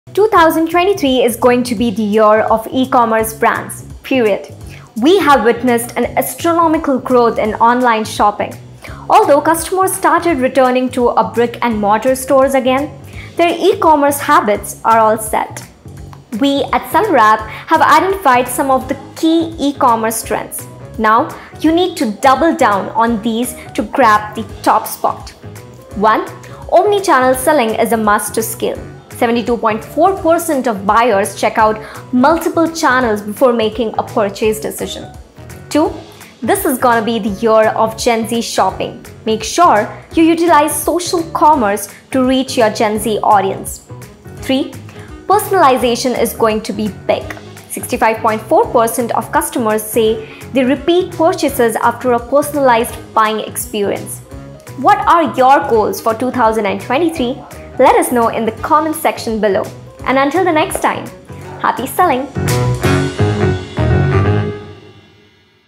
E-commerce Trends That Will Help You Succeed in 2023
2023 is going to BE the year of E-commerce brands. Period!

Global E-Commerce Sales will reach a COLOSSAL of $6.3 Trillion!

Ankita Patil, Product Evangelist at SellerApp, is here to tell you some of the key eCommerce trends that you NEED to DOUBLE DOWN to grab the top spot!